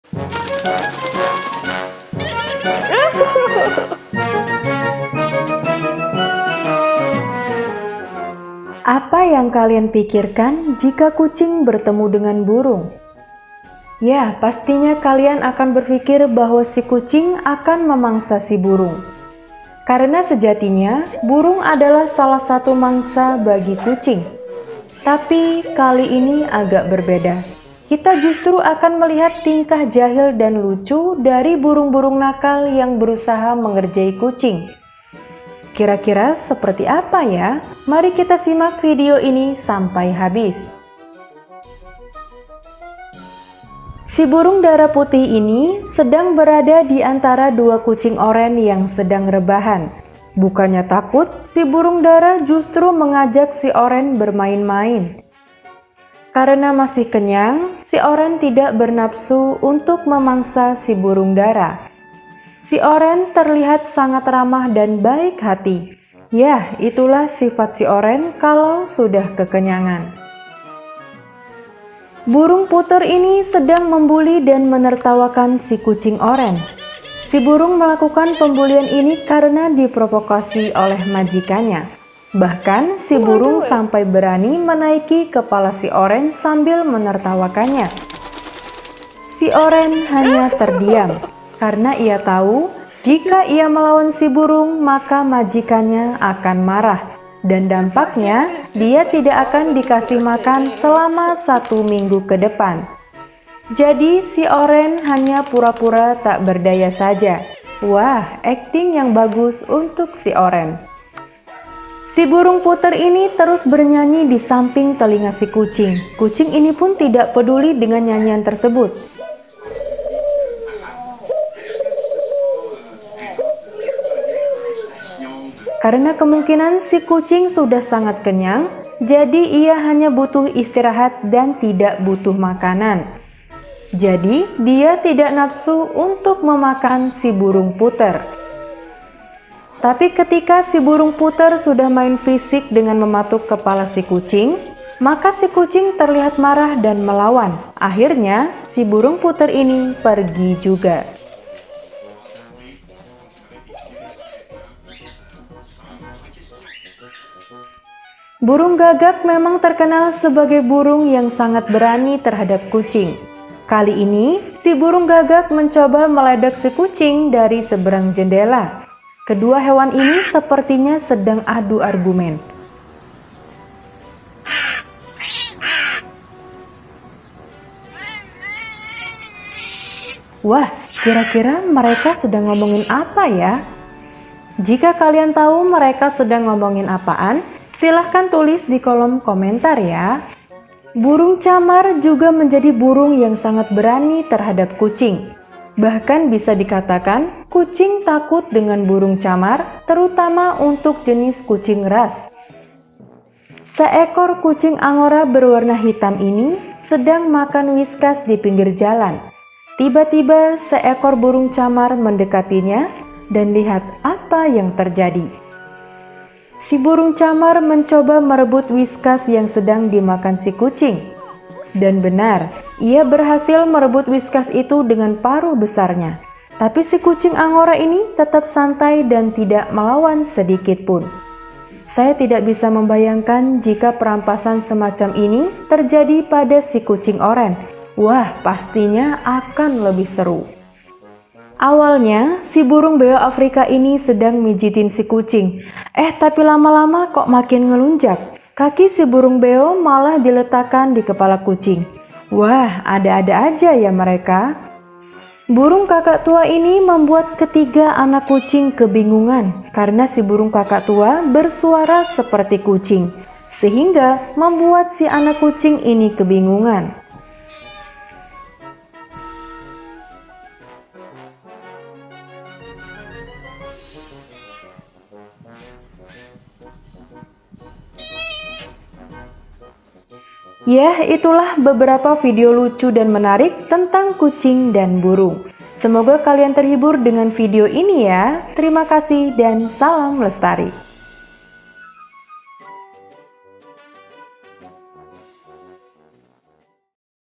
NGAKAK!! Momen Kocak Kucing Oren Bar-bar Ketakutan Dipatok Burung! Kucing VS Burung Gagak
NGAKAK!! Momen Kocak Kucing Oren Bar-bar Ketakutan Dipatok Burung Dara! Kucing VS Burung

Our contact wrote in our youtube description. Thank you